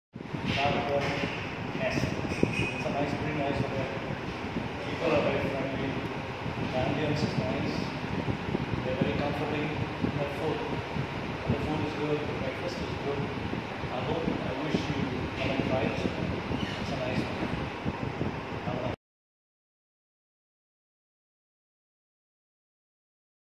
Prabir Poddar at FabExpress Nest, Kolkata | Guest Review
Prabir Poddar talks about his experience of staying at FabExpress Nest, Kolkata.